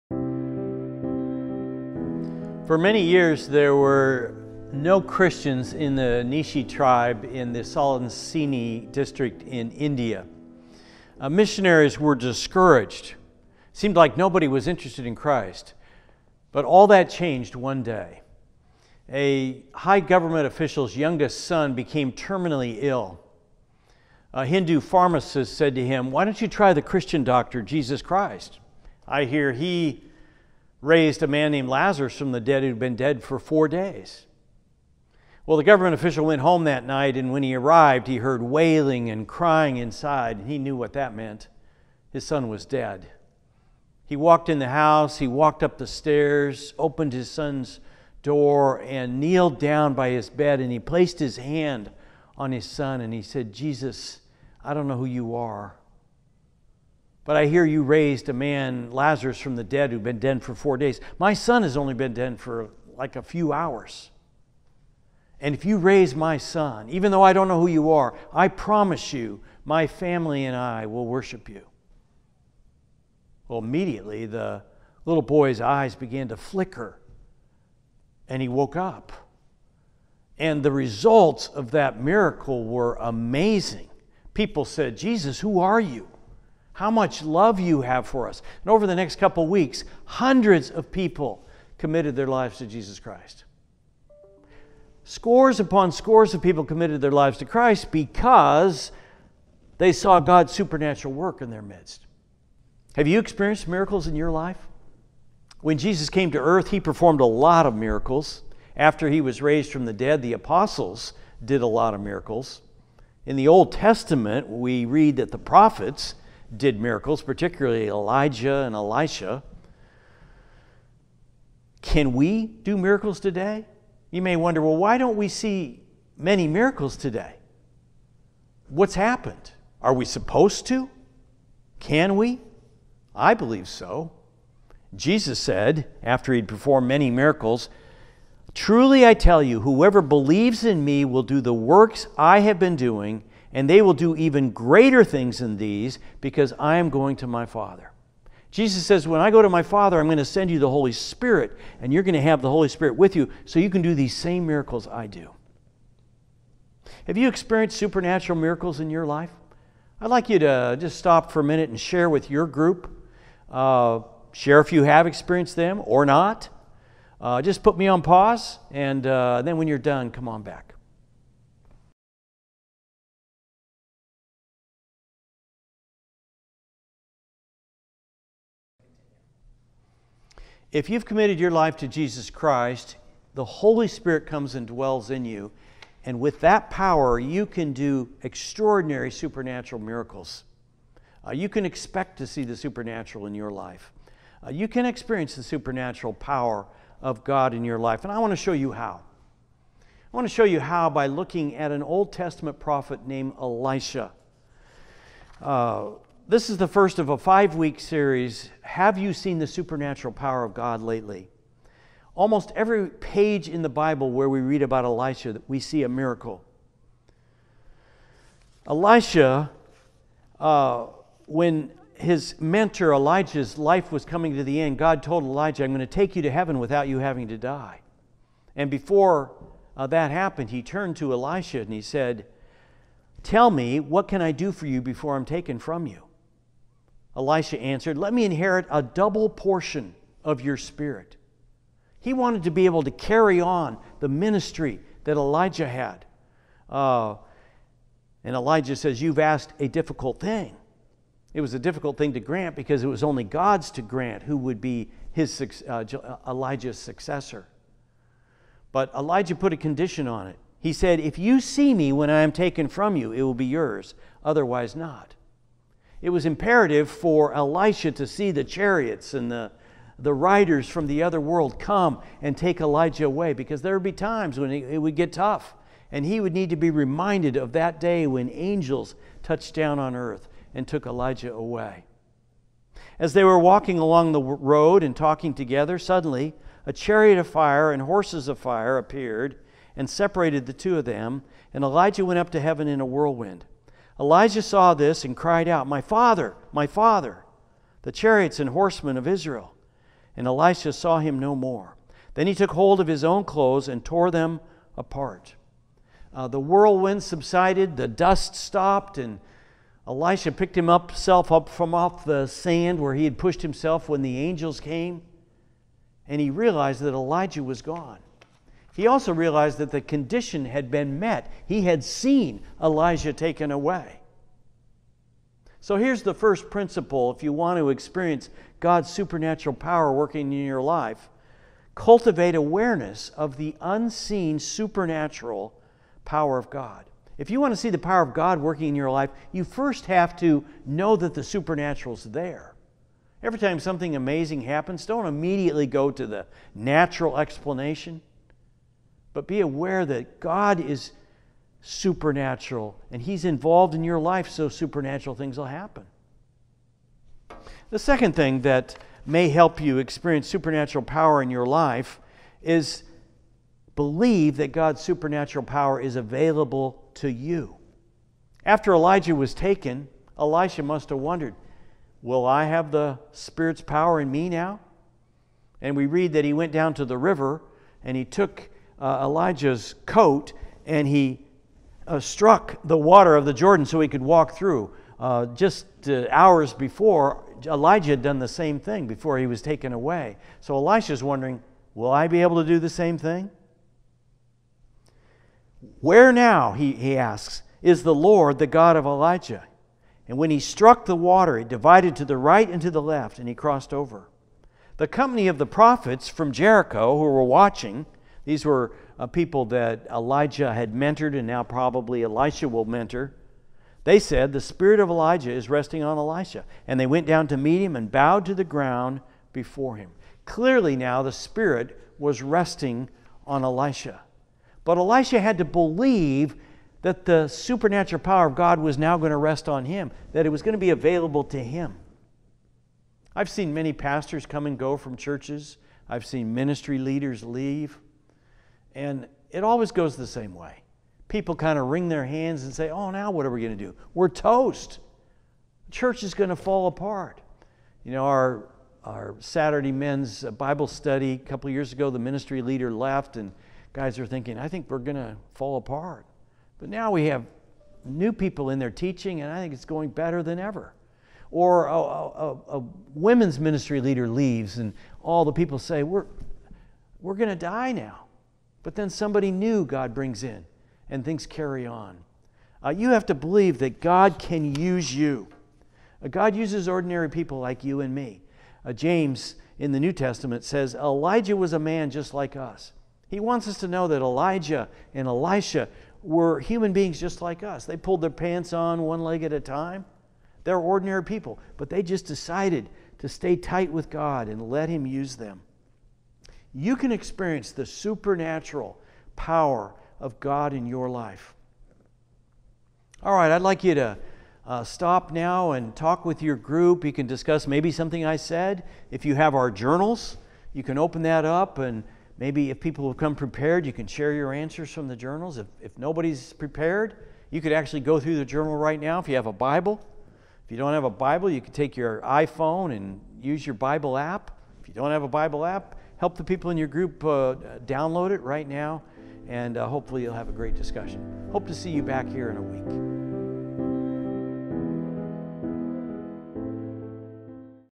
Have You Seen God's Supernatural Power Lately - Week 1
Do miracles happen today?  Join us for Portland Community Church's 5-week study on God's supernatural power, taught by Ron Kincaid and beginning April 8th, 2018.